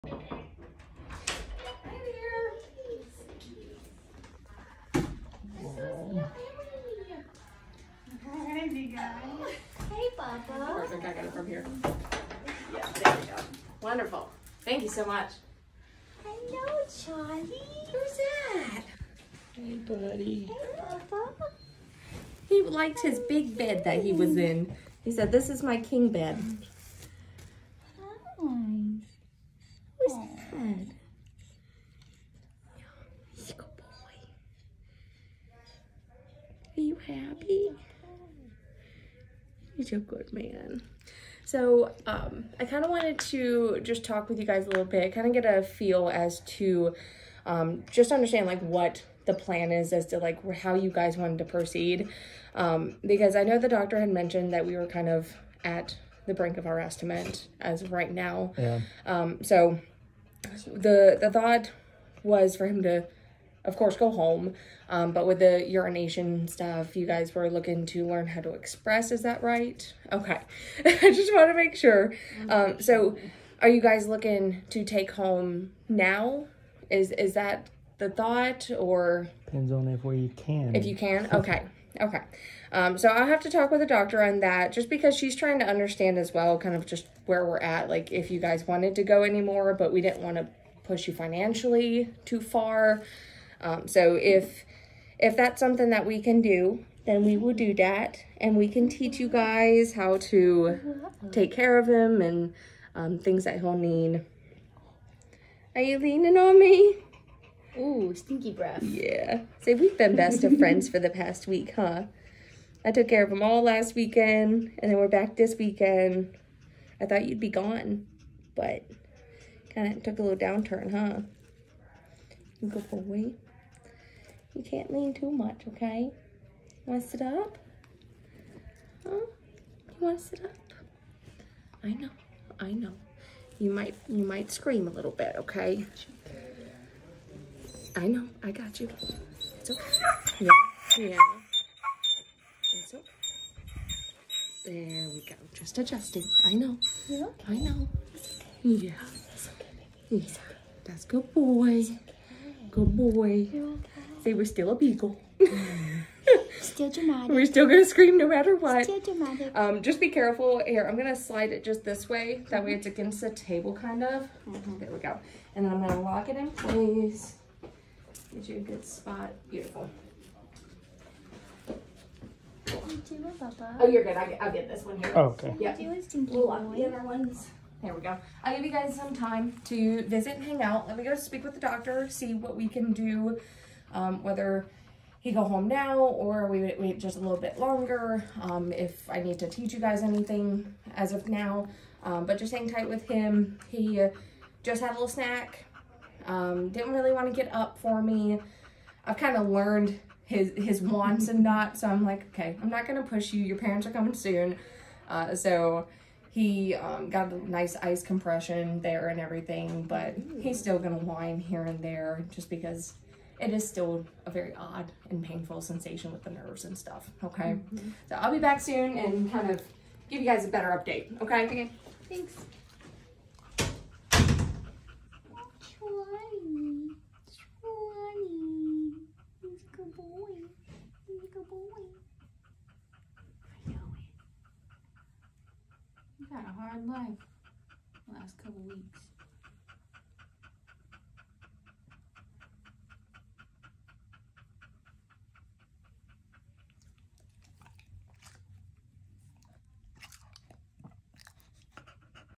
UPDATE #9 visit #2
It’s hard to see your furry friend in such shape but, if your pup is in a hospital please visit them. It lets them know you are still there and care and that you have not given up so they should not as well.
Keep fighting Charlie we all care and hope you get home soon.
A benefit for Charlie to help cover the cost of the operations . There will be a live band, “Parrot Party Band” and will be 7pm till 10pm on June 7th.